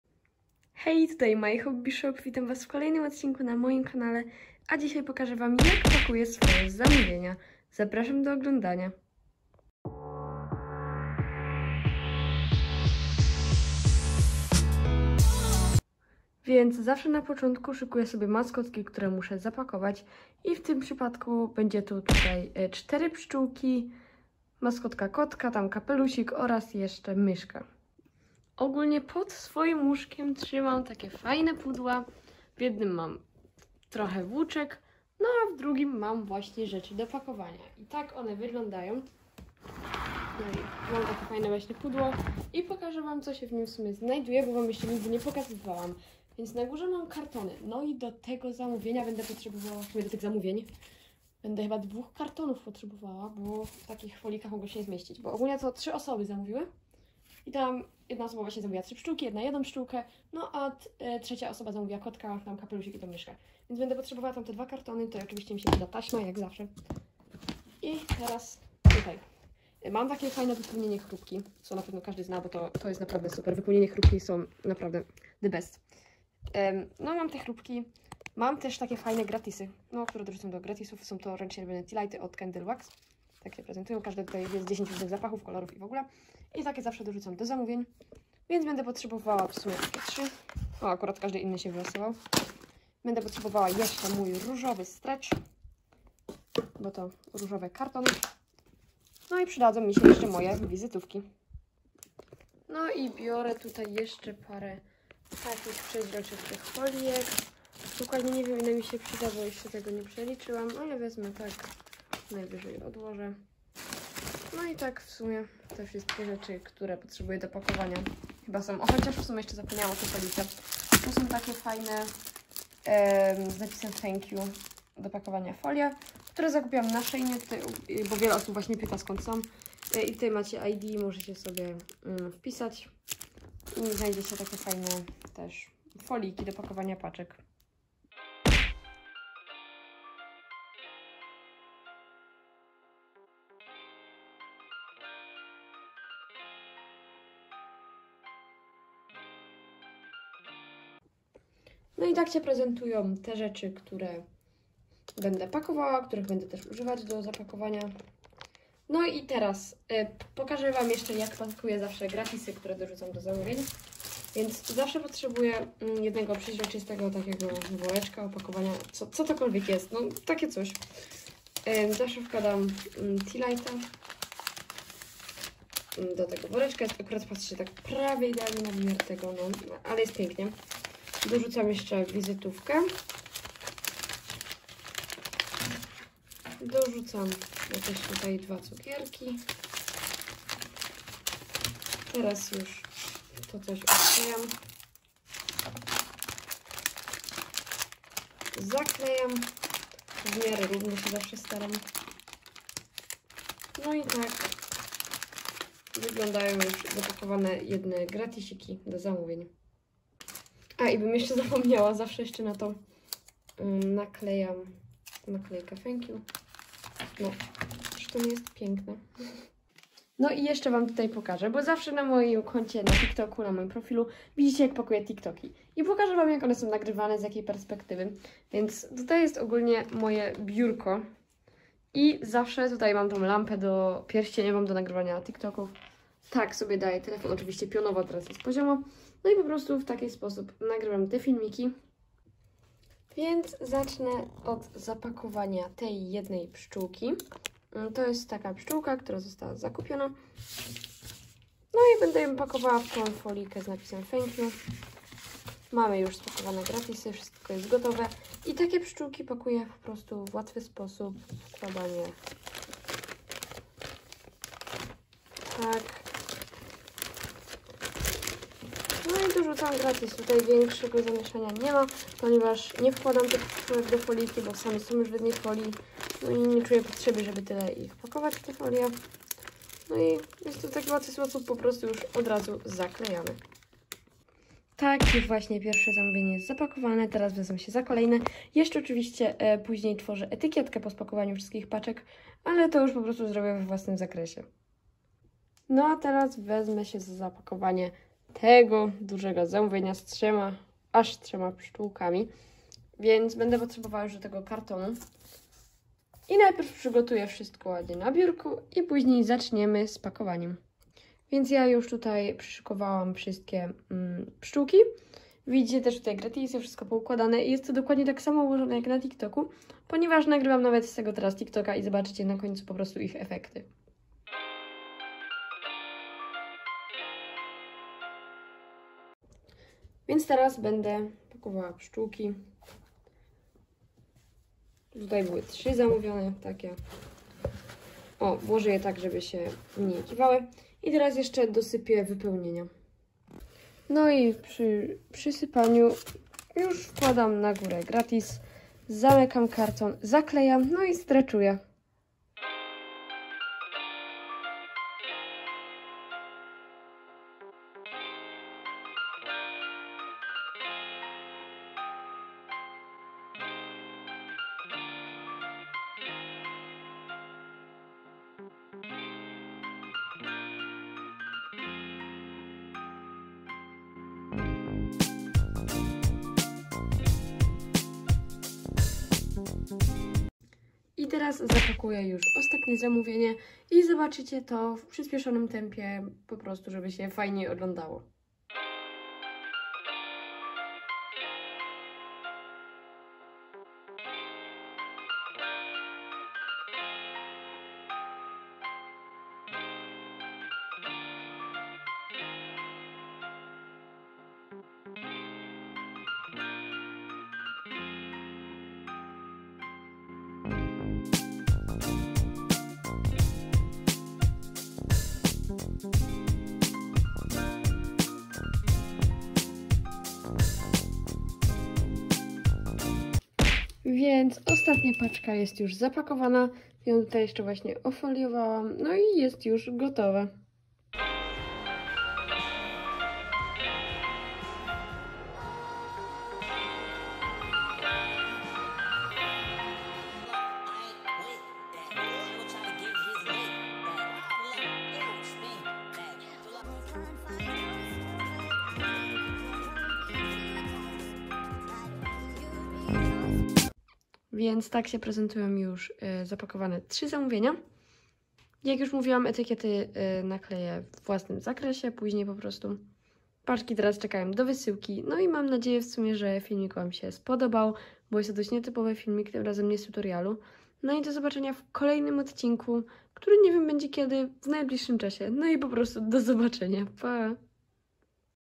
Jak pakuję swoje zamówienia? | Pakowanie oczami sprzedawcy
Hejka!
W dzisiejszym filmie pokazałam wam jak wygląda pakowanie moich zamówień, oczami sprzedawcy
koniecznie zostawcie łapkę w góre oraz subika na kanale :)
moje sociale